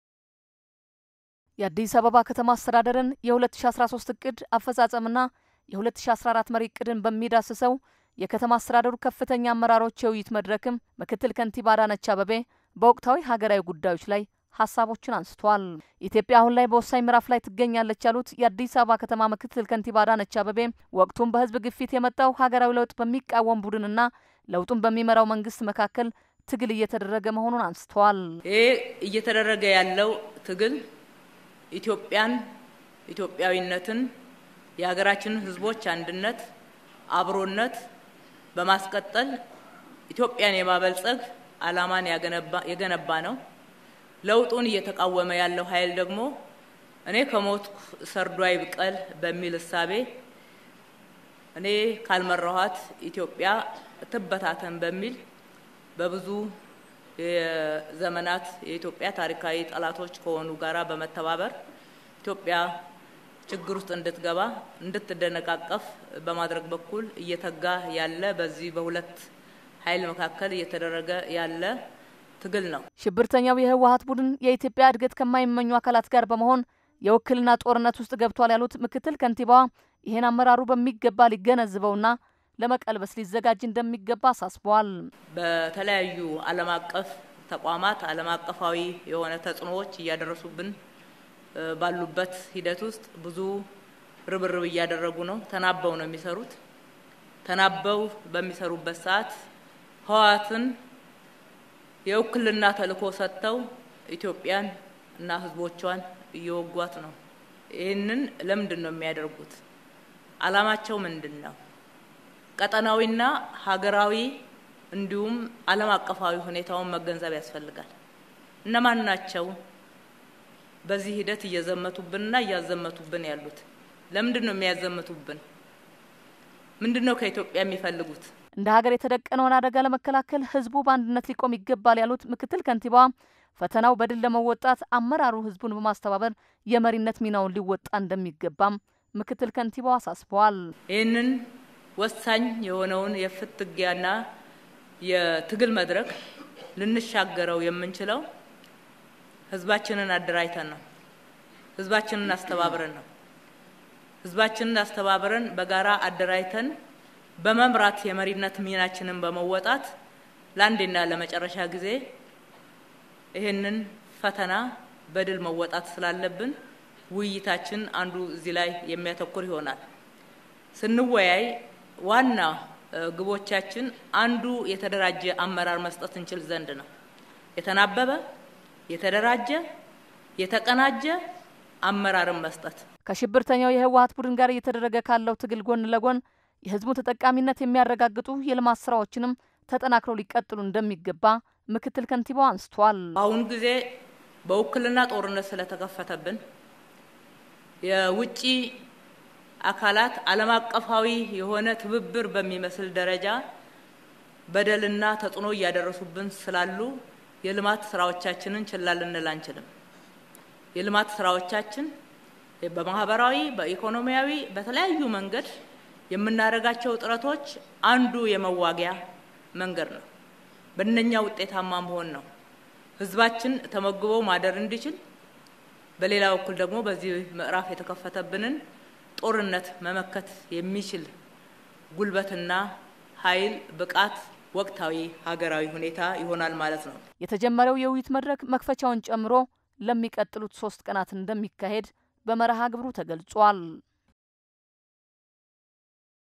ኢትዮጵያ ያጋጠማትን ፈተና ለመወጣት አመራሩ ሚናውን ሊወጣ ይገባል (ሐምሌ 28/2013 ዓ.ም)
ኢትዮጵያ አሁን ላይ ያጋጠማትን ፈተና በድል ለመወጣት አመራሩ ህዝቡን በማስተባበር  የመሪነት ሚናውን ሊወጣ እንደሚገባ የአዲስ አበባ ከተማ ምክትል ከንቲባ አዳነች አቤቤ ገለጹ፡፡
ከሽብርተኛው የህወሃት ቡድን ጋር እየተደረገ ካለው ትግል ጎን ለጎን የህዝቡን ተጠቃሚነት የሚያረጋግጡ የልማት ስራዎችንም ተጠናክረው ሊቀጥሉ እንደሚገባ ምክትል ከንቲባዋ አንስተዋል፡፡